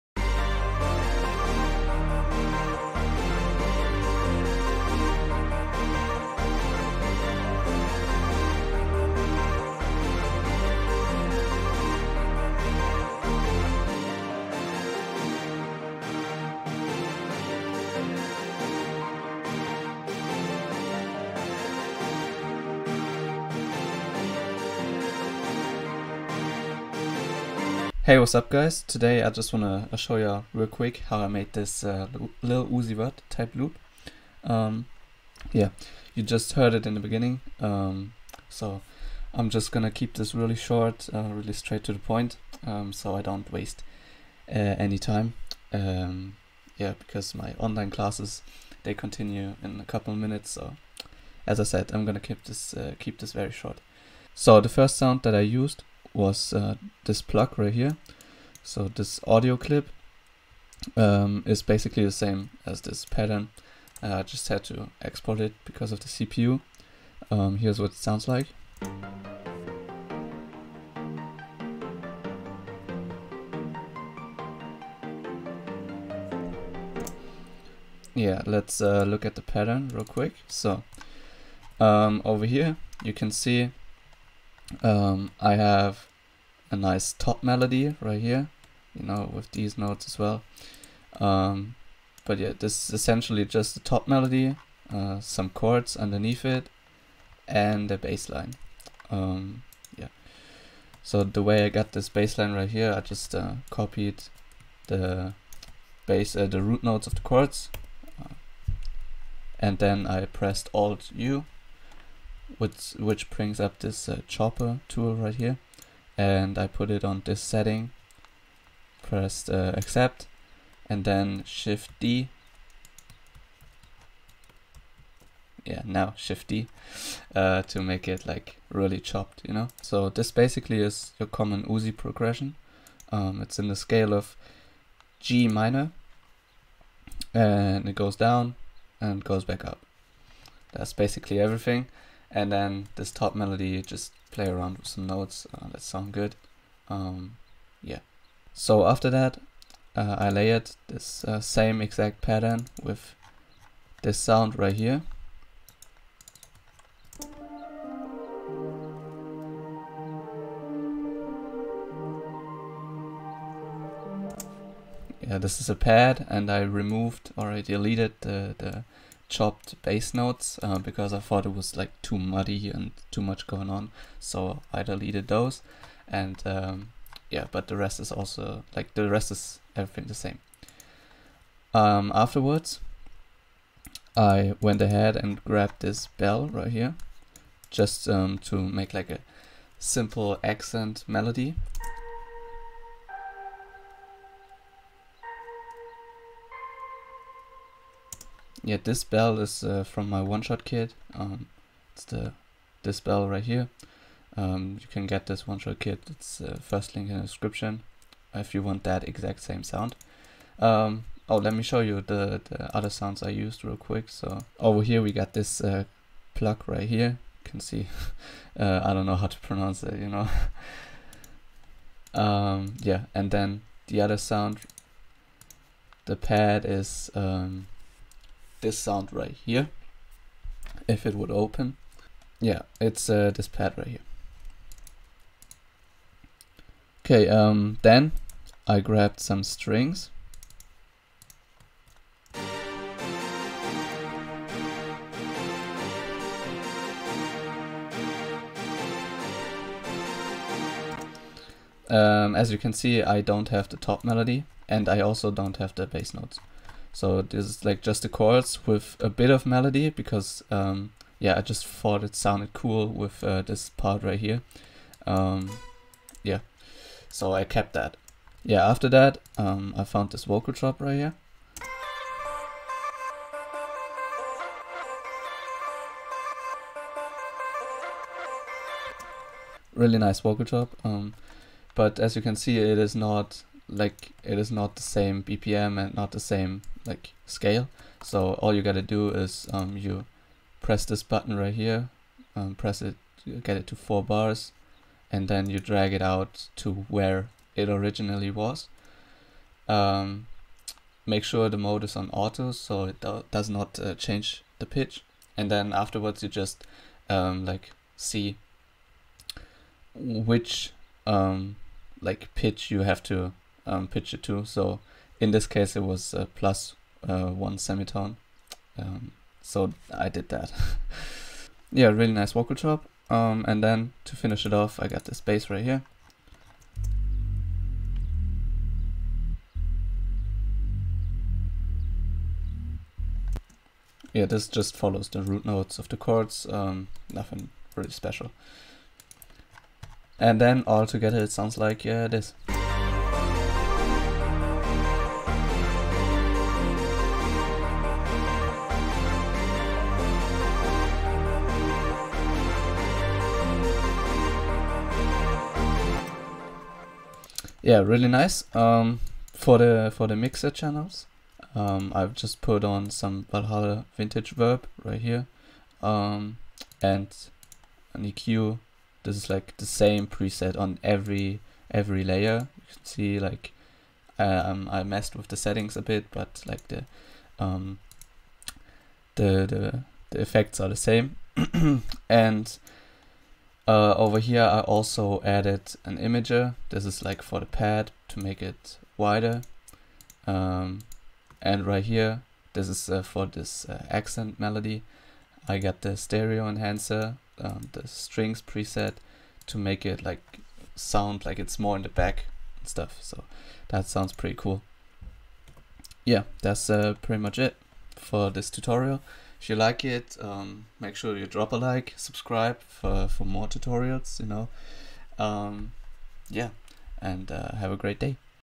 How To Make Fire Glo Melodies For LIL UZI VERT | Orchestral Lil Uzi Vert Type Melody Loop Tutorial
In this tutorial I'm showing you how to make orchestral lil uzi vert type loops in FL Studio. This Lil Uzi Vert Type Melody could be used for his upcoming album! This is a new lil uzi vert glo melody tutorial on how to make some fire melodies for lil uzi vert!